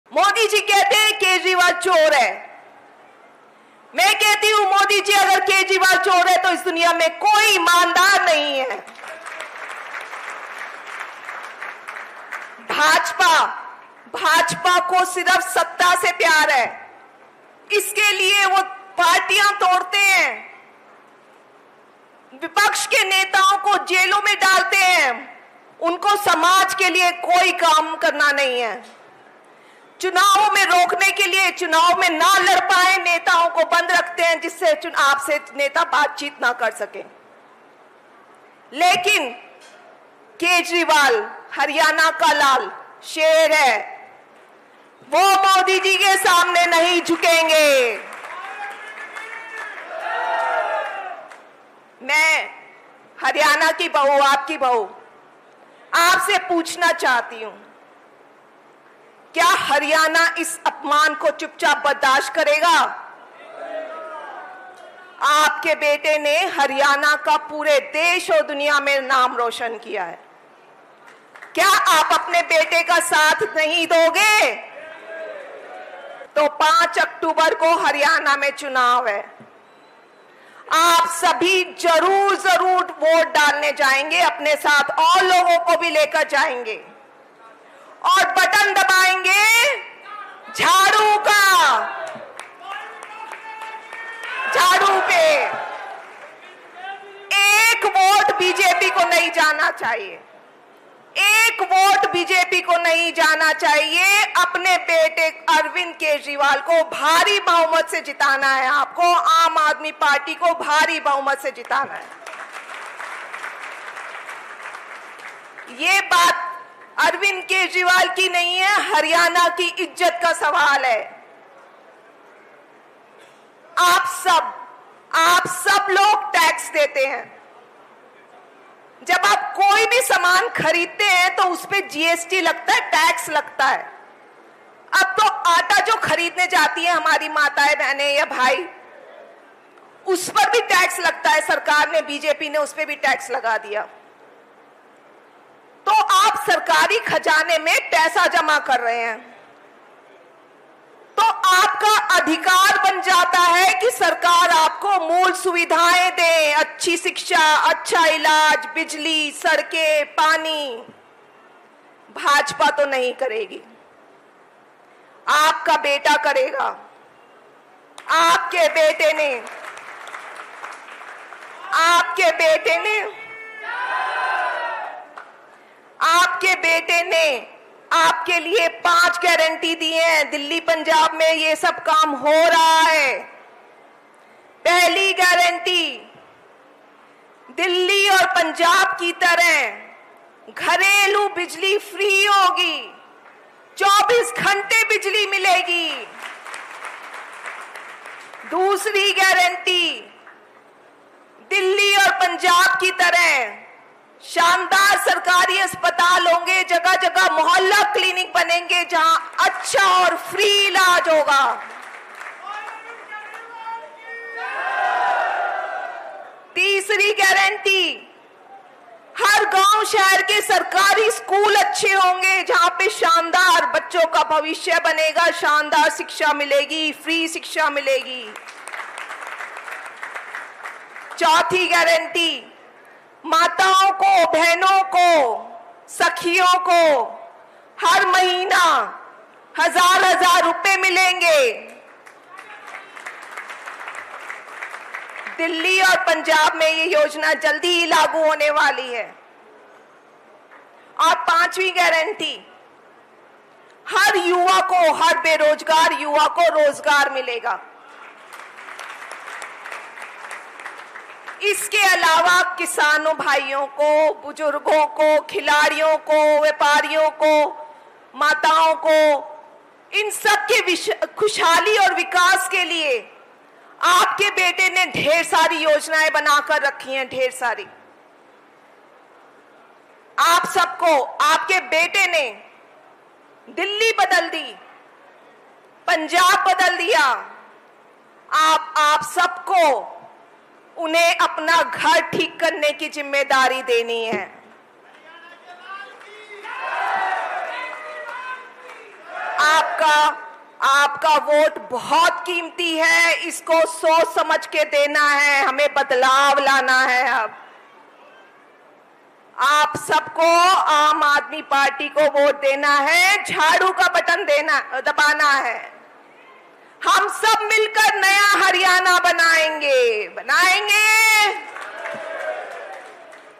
Arvind Kejriwal का नाम लेकर PM Modi पर बरस पड़ीं Sunita Kejriwal, सुनिए क्या बोलीं?।Haryana Politics
सुनीता केजरीवाल ने महम में जनसभा को संबोधित किया और कहा कि दिल्ली के मुख्यमंत्री अरविंद केजरीवाल के 'हरियाणा के लाल' हैं, उन्होंने कहा कि फर्जी मामले में वे सलाखों के पीछे हैं, वो शेर हैं और वह पीएम मोदी के सामने झुकेंगे नहीं.

Sunita Kejriwal addressed the public meeting in Meham and said that Delhi Chief Minister Arvind Kejriwal is the 'Lal of Haryana', she said that he is behind the bars in a fake case, he is a lion and he will not bow down before PM Modi.